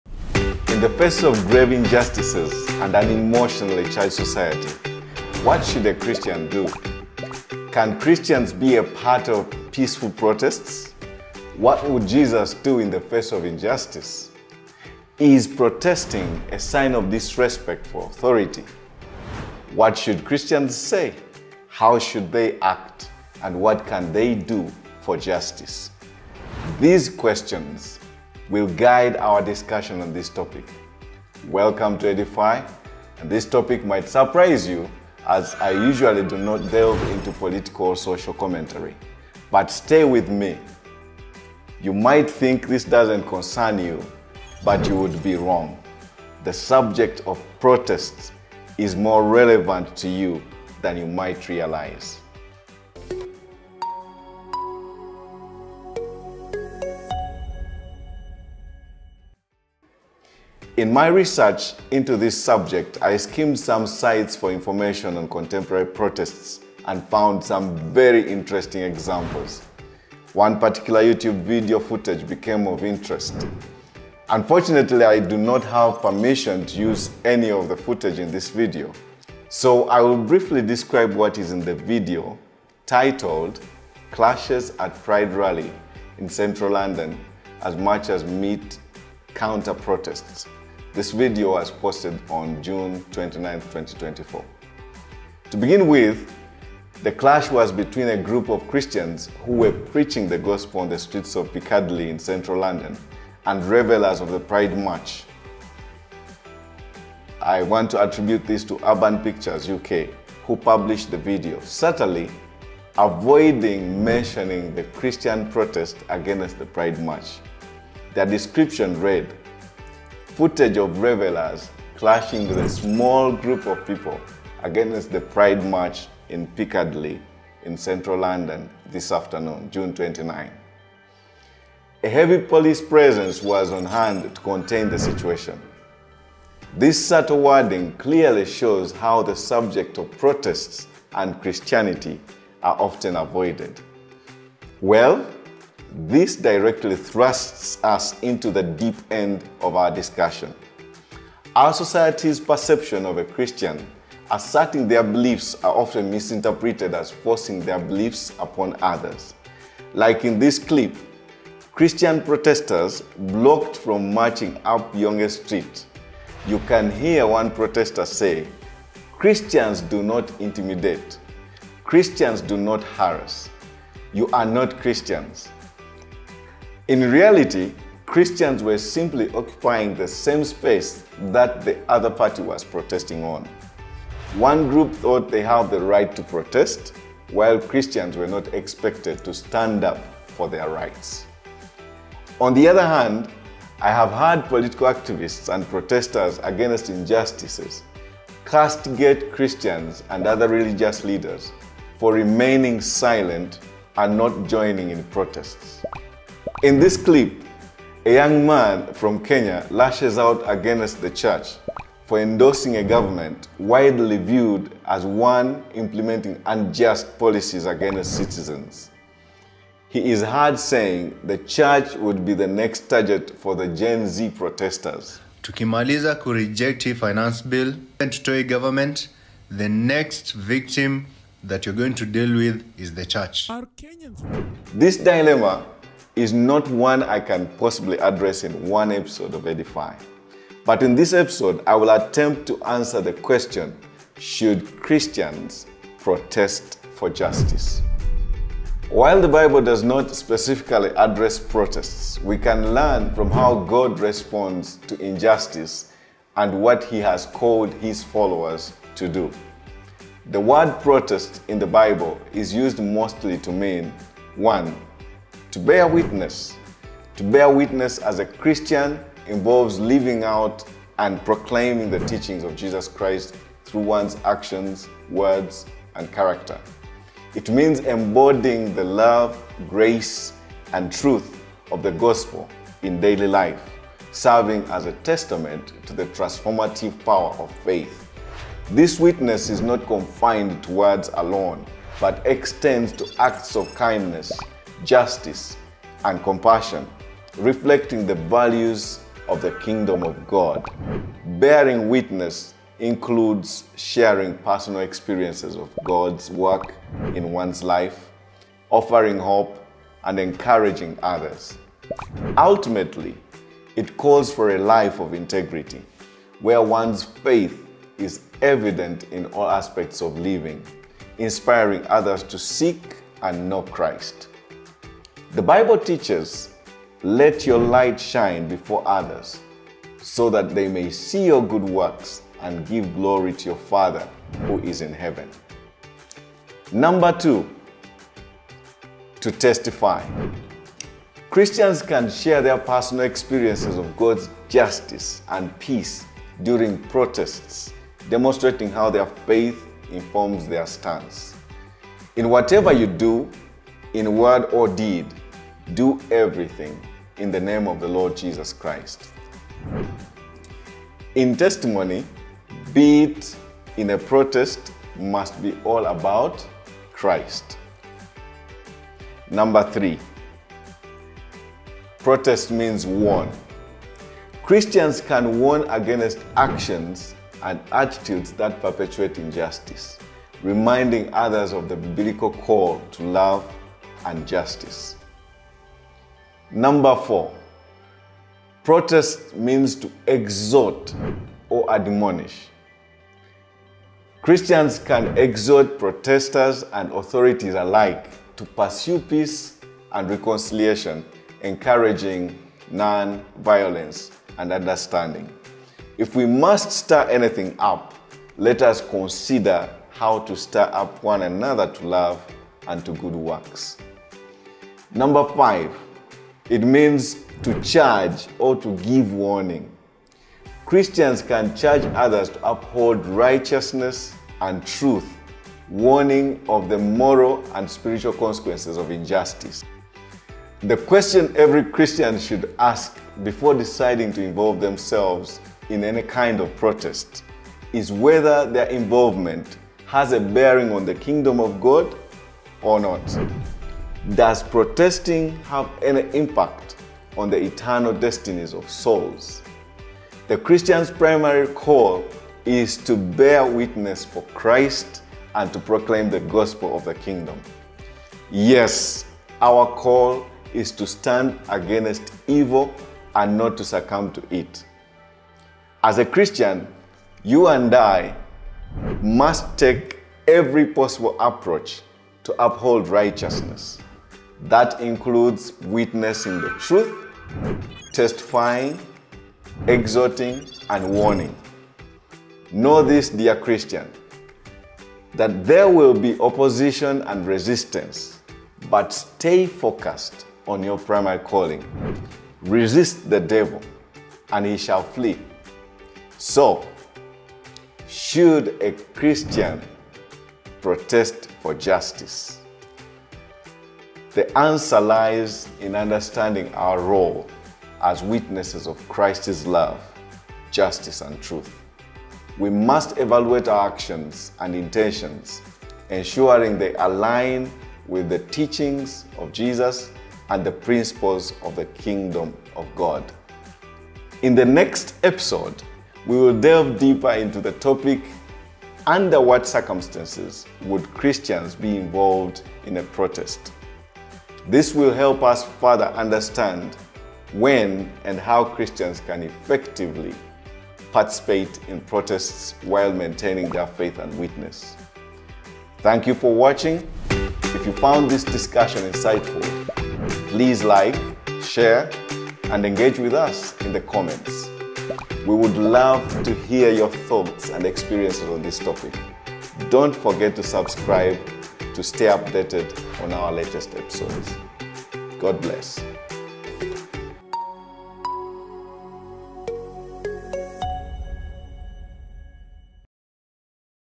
Should Christians Protest for Justice
While the Bible does not specifically address protests, we can learn from how God responds to injustice and what He has called His followers to do. This video series provides biblical perspective to the subject of protests and the Christian's role in a super-charged volatile culture and environment.